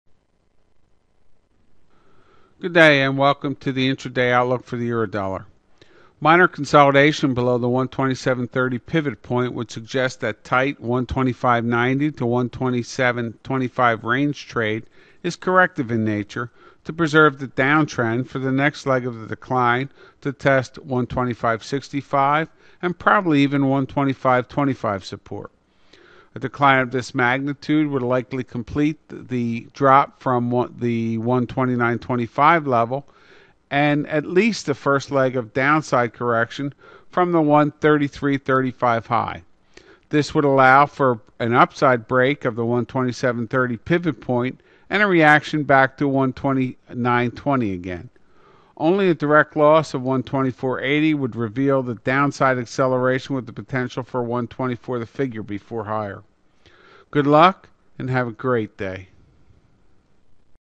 Houlry Forex Signal for Euro, Aug 25th, 2010 22gmt
The erratic upflag since the 83.60 low to again fail the key 84.75-85 resistance area suggest there is still risk for a final new low of about 83.20. From this lower level there is a risk of a larger correction to the entire 89.15 to 83.20 decline. Thus stay short term bearish against the 85.10 overlap level. Considering we are still in long term downtrend there is scope for this decline to extend but as 83.00-20 support holds we would expect a larger upside correction to develop.